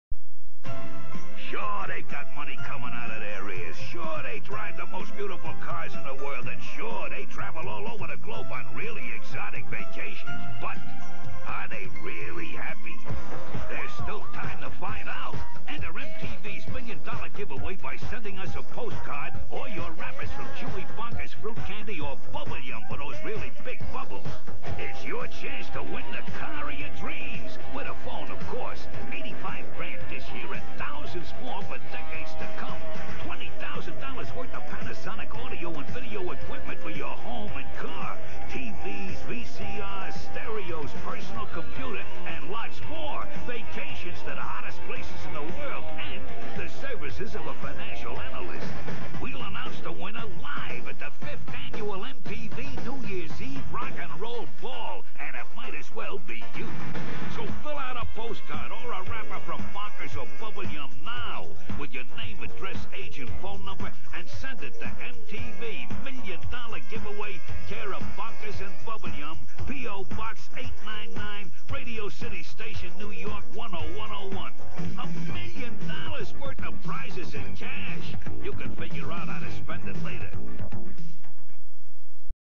1985 MTV Million Dollar Giveaway Commercial
Aired December of 1985 during Heavy Metal Mania.